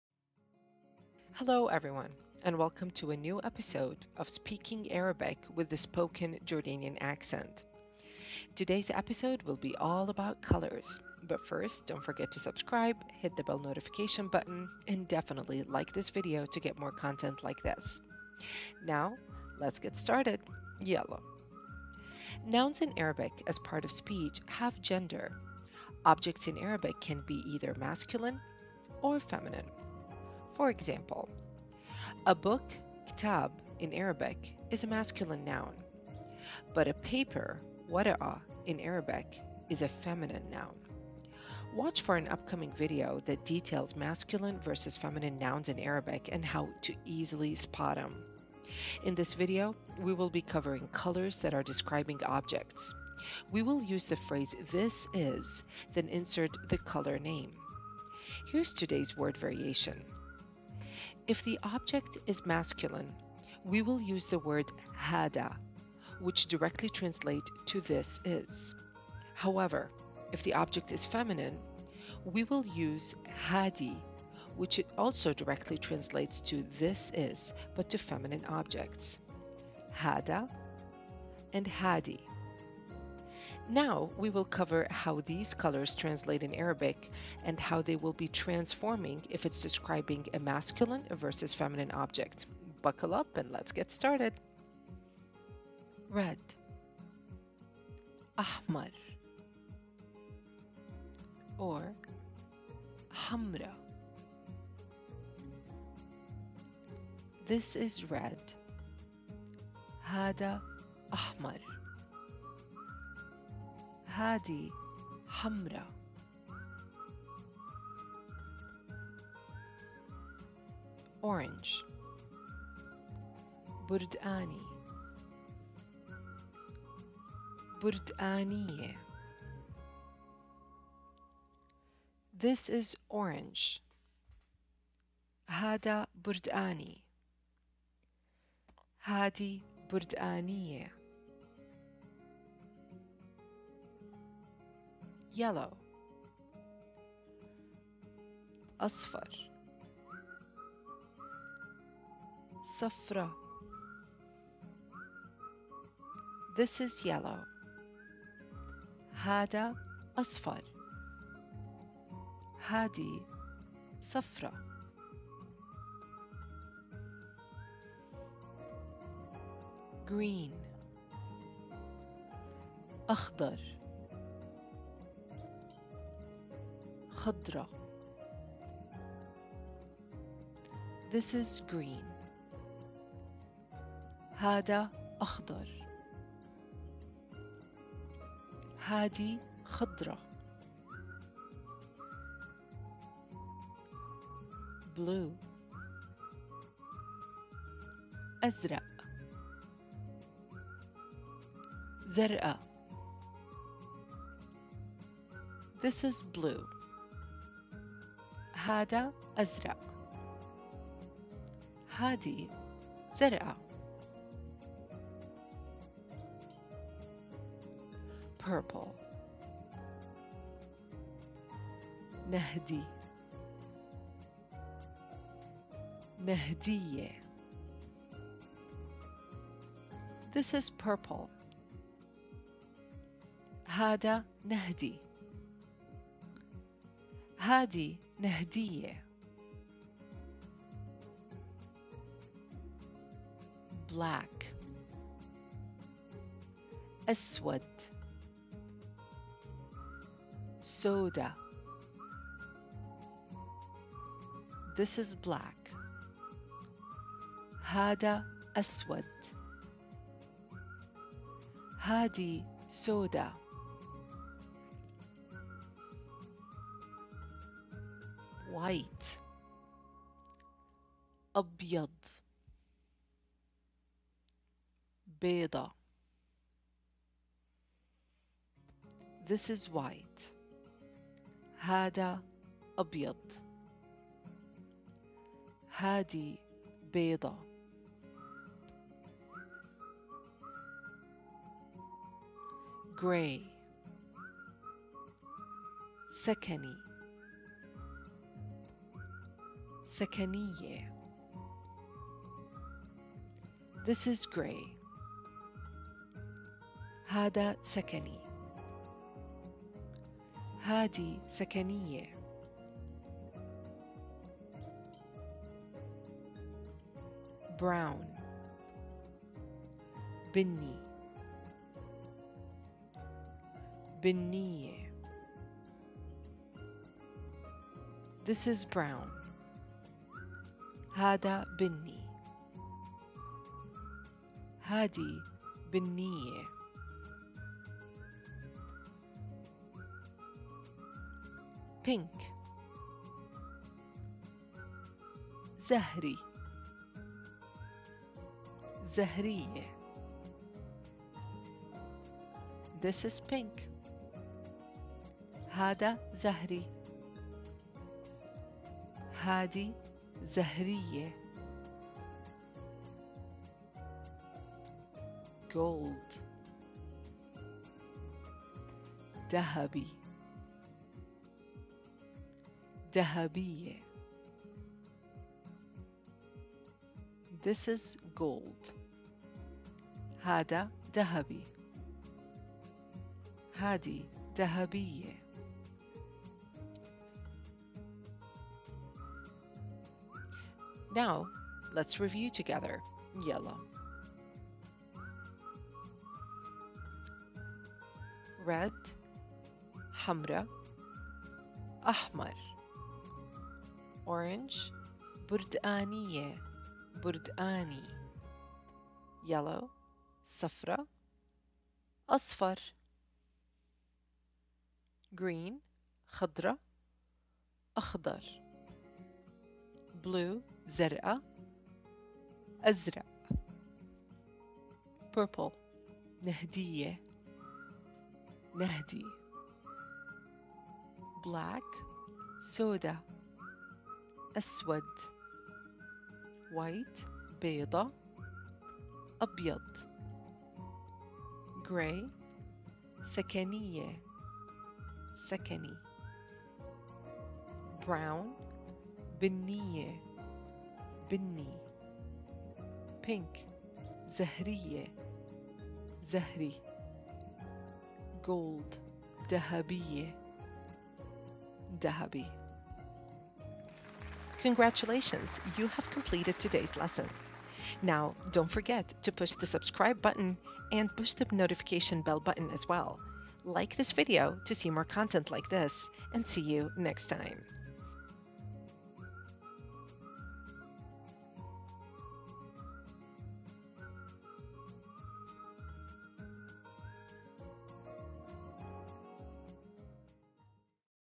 ARABIC BASICS - Colors & its variations - practical tips - Jordan Accent - Speak Arabic Like a Pro
Episode 4 - Arabic Basics - today's lesson is about colors and its variations describing both masculine and feminine objects - one of a kind lesson! Can you learn all these colors? Spoken in Jordanian accent - Enjoy!